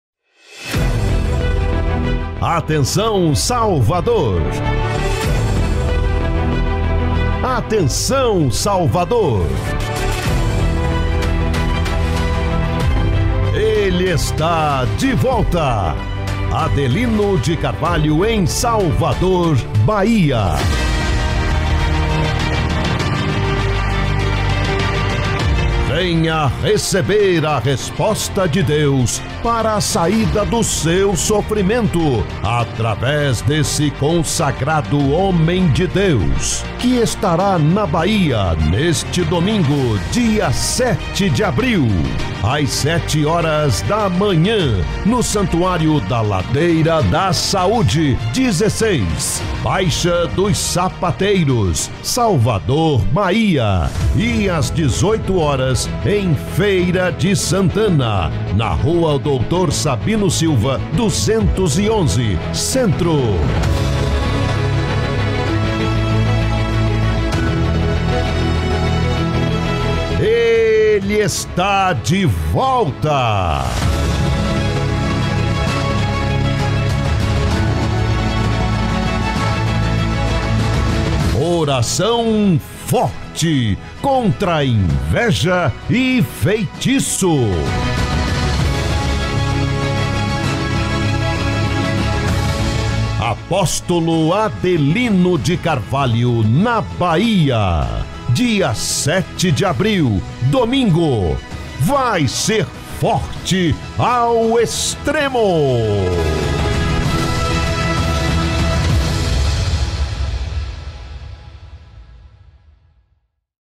Apóstolo Adelino de Carvalho em Salvador/BA
ELE ESTÁ DE VOLTA!!!
ADELINO DE CARVALHO EM SALVADOR/BA

Venha receber a resposta de Deus para saída do seu sofrimento através desse consagrado homem de Deus, que estará na Bahia, neste domingo, dia 7 de abril

Às 7 horas da manhã no Santuario da Ladeira da Saude, 16 – baixa dos Sapateiros - Salvador/BA
E as 18 horas, em Feira de Santana, na rua Doutor Sabino Silva, 211 – centro.

ORAÇÃO FORTE CONTRA INVEJA E FEITIÇO.
VAI SER FORTE AO EXTREMO!